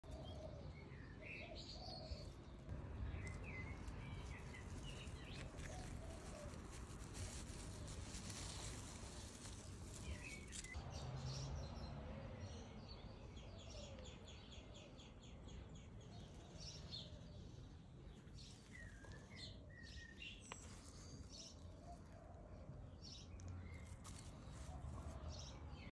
Green tree frog let’s me pick him up
Australia native green tree frog